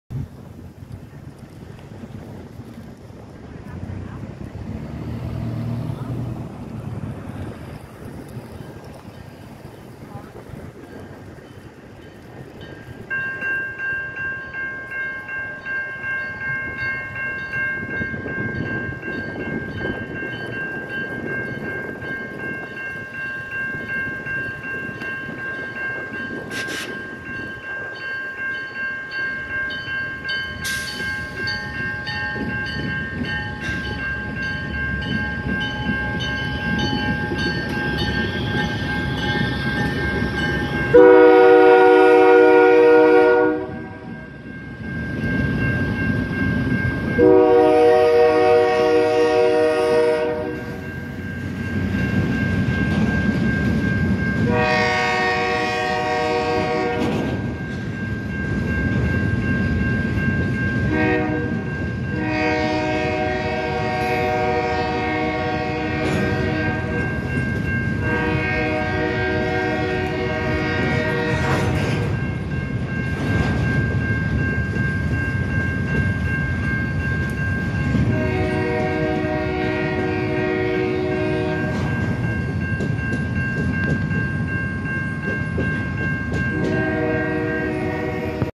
CN Freight Train In Vernon , BC , Canada 2021
The Canadian National Railway Train provides a southbound run on the Okanagan and Lumby Subdivisions.  Here I catch CN2607, CN2681 and CN2532 with a mix of lumber and wood pellet cars making their way through downtown Vernon B.C.  This was located at 39th Avenue grade railroad crossing where Mile 85.00 Okanagan SUB.  This was recorded on October 24, 2021.